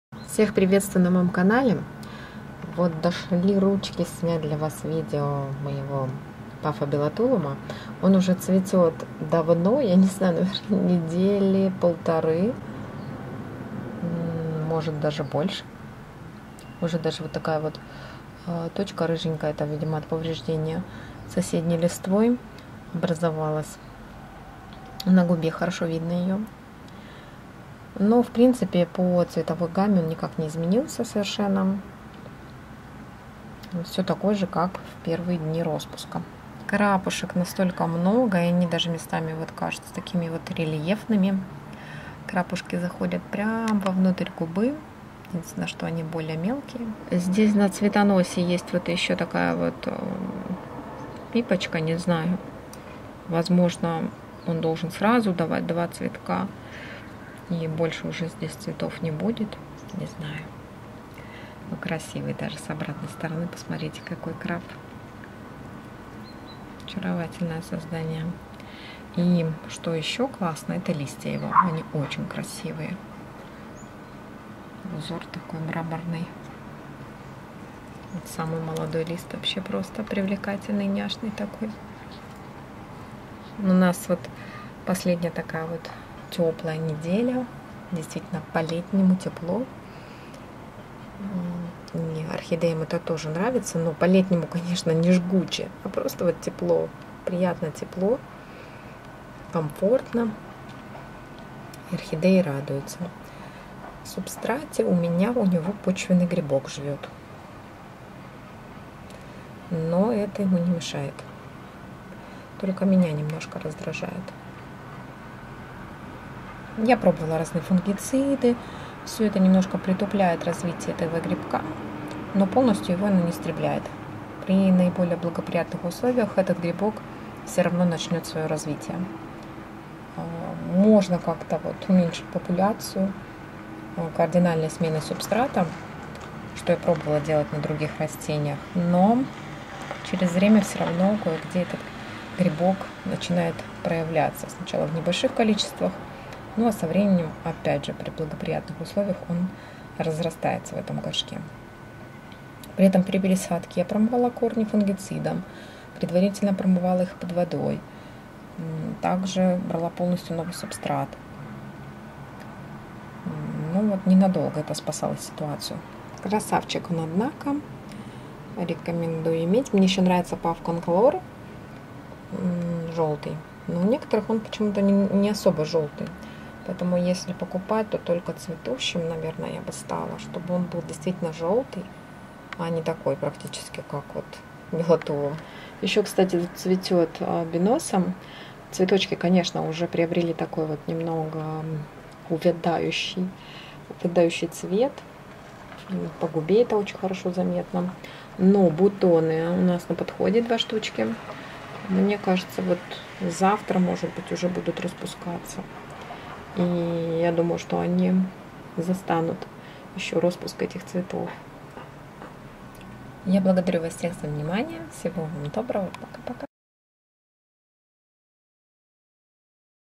Цветет впервые пафиопедилум Беллатулум и продолжает брассокаттлея Биноса
Масла для тела, волос и кожи (жожоба, персиковой косточки, виноградной косточки, касторовое и прочие) Арахисовая паста на выбор